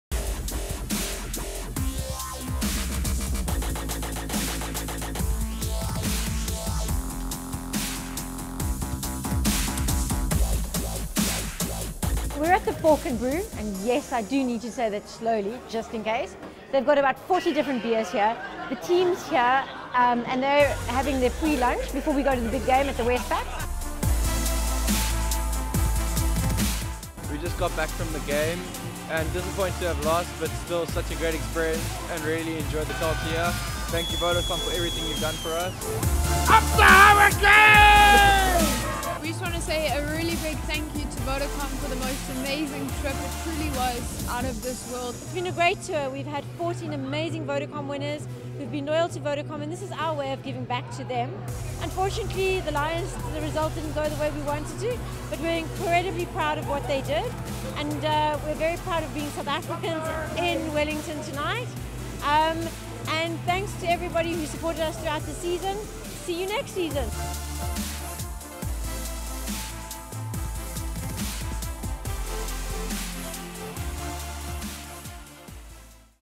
Vodacom Super Rugby Final Tour - All Out Rugby - Day 4
Match Day in Wellington and the title was claimed by the Hurricanes, but the Vodacom Super Rugby Tour winners had a blast. 2016 gave us a new format, more teams, exciting play, the magnificent rise of the Emirates Lions and a whole lot more. We can't wait for Vodacom Super Rugby 2017!